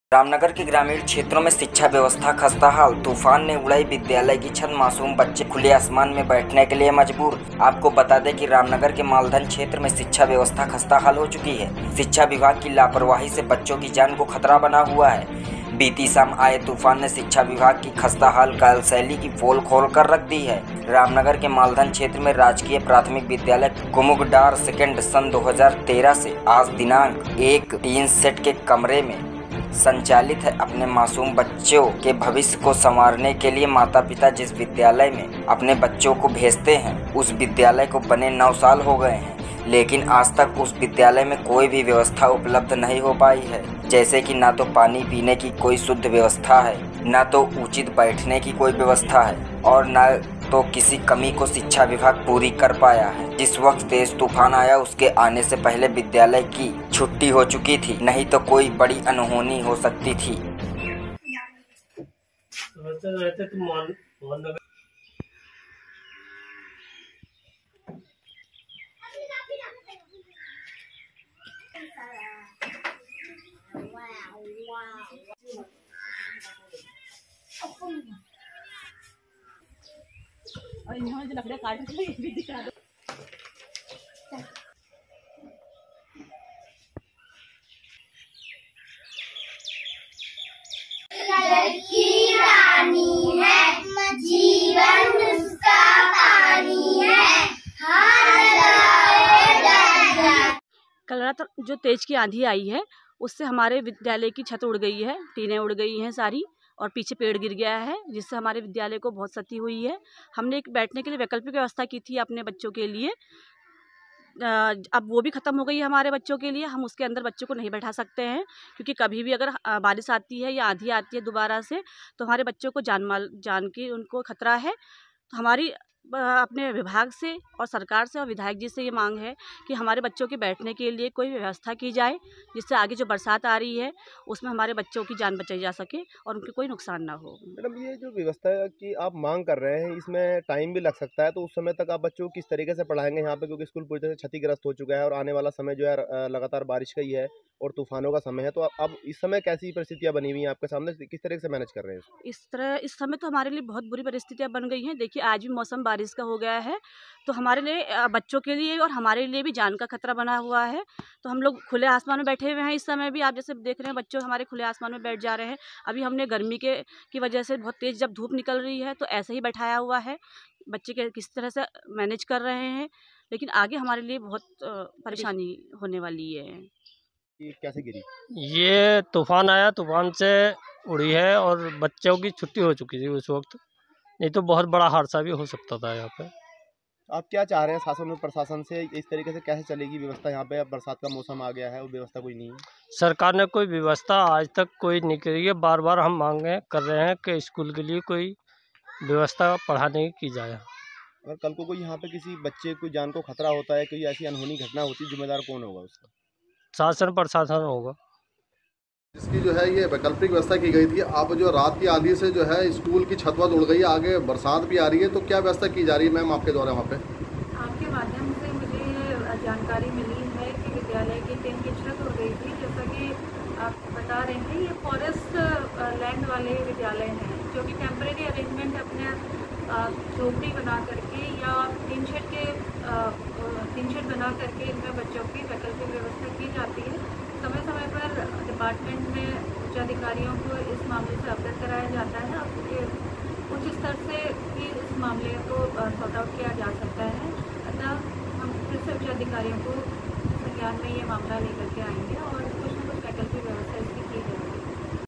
रामनगर के ग्रामीण क्षेत्रों में शिक्षा व्यवस्था खस्ताहाल
तूफान ने उड़ाई विद्यालय की छत मासूम बच्चे खुले आसमान के नीचे बैठने को मजबूर है आपको बता दे रामनगर के मालधन क्षेत्र में शिक्षा व्यवस्था खस्ता हाल हो चुकी हैं। शिक्षा विभाग की लापरवाही से बच्चों की जान को खतरा बना हुआ है। बीती शाम आये तूफान ने शिक्षा विभाग की खस्ता हाल कार्यशैली की पोल खोल कर रख दी हैं। रामनगर के मालधन क्षेत्र में राजकीय प्रथमिक विद्यालय कुमुगडार सेकेंड सन 2013 से आज दिनांक तक एक टीन सेट के कमरे में  संचालित है। अपने मासूम बच्चों के  भविष्य को सबारने के लिए माता पिता जिस विद्यालय की आस लगा कर बच्चों को पढ़ने के लिए भेजते है। उस विद्यालय को बने नो साल हो गए है। लेकिन आज तक ना तो बच्चों के बैठने की उचित व्यवस्था हुई हैं। ना बच्चों के लिए पीने का पानी की कोई व्यवस्था शिक्षा विभाग ओर शासन करवा पाया हैं। जिस वक्त तेज तूफान के आने से विद्यालय की टीन उड़ी गनीमत रही कि  विद्यालय की छूट्टी हो चुकी थीं नही तो कोई बड़ी अनहोनी हो सकती थी।